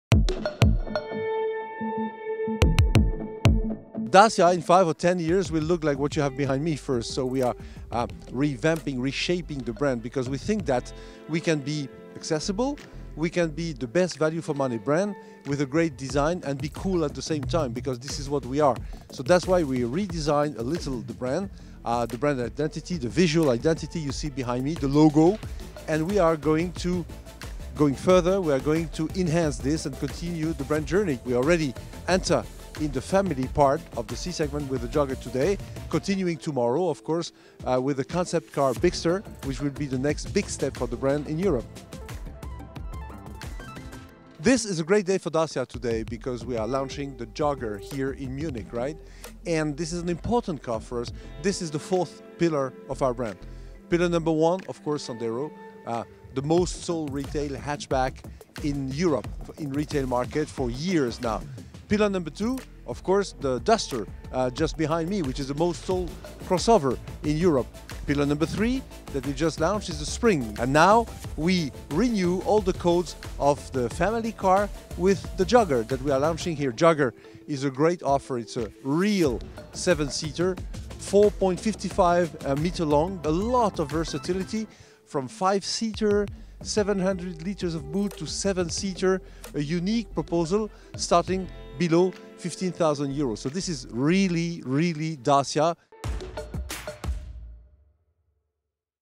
Interview de Denis Le-Vot, Directeur Général Dacia | Renault Group
Denis Le-Vot est interviewé à propos de la nouvelle identité de marque Dacia, du renouvellement entamé de la gamme et du Nouveau Dacia Jogger tout juste dévoilé lors du Salon automobile IAA de Munich.